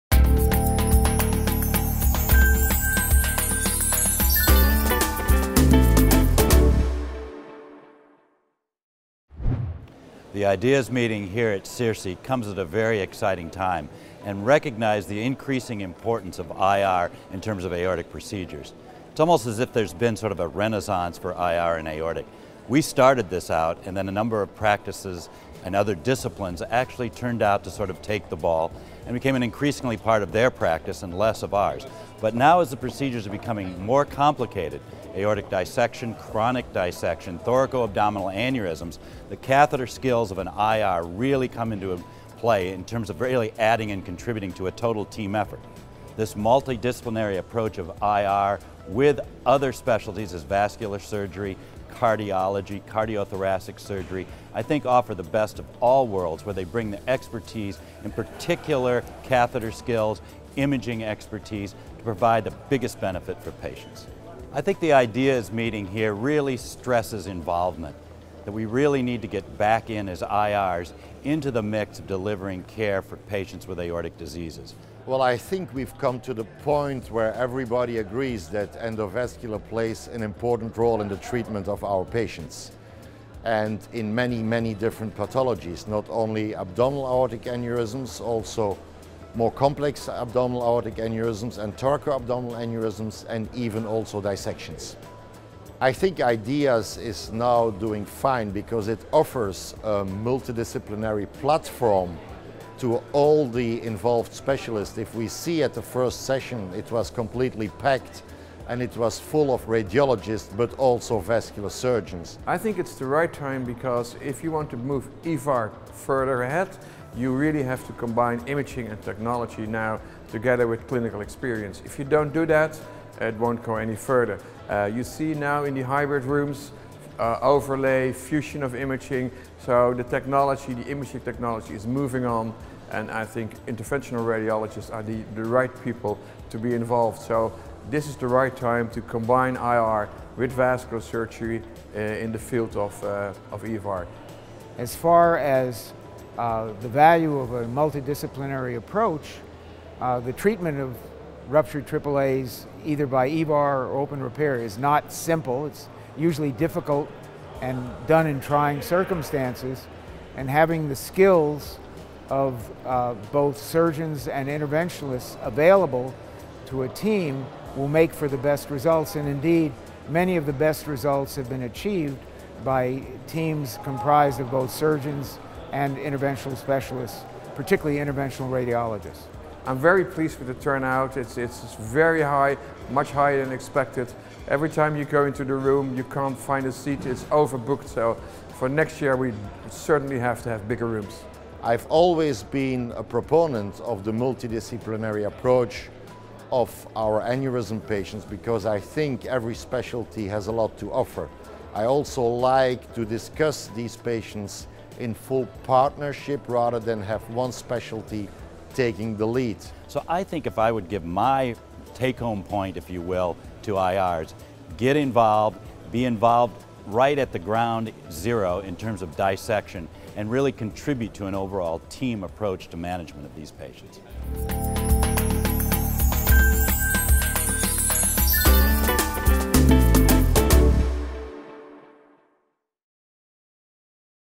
Introducing IDEAS: The new endovascular aortic symposium
During CIRSE 2015 in Lisbon, a parallel symposium dedicated to endovascular aortic treatment was held for the first time. This symposium welcomed a multidisciplinary faculty and audience, and those attending CIRSE 2015 benefitted from unrestricted access.

Key faculty members Frank Veith, Jim Reekers, Eric Verhoeven and Mike Dake explain why the time is right for truly interdisciplinary aortic education and exchange.